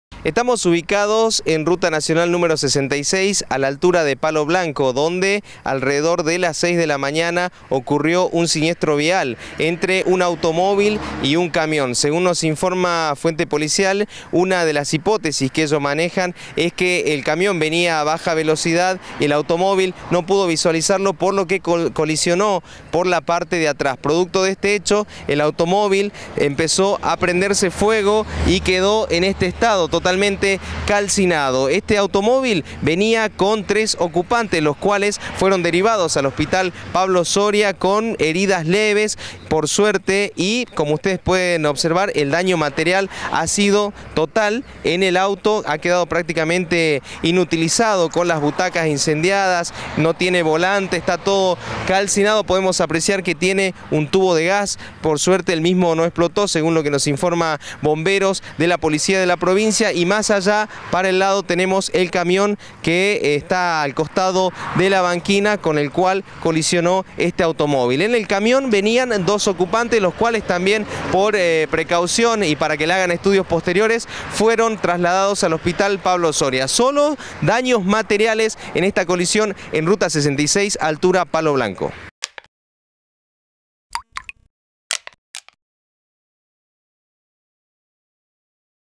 Un vehículo se incendió esta mañana sobre la Ruta Nacional 66
Sus tres ocupantes resultaron heridos. Según la hipótesis de la policía, habría impactado desde atrás contra un camión. Mirá el video.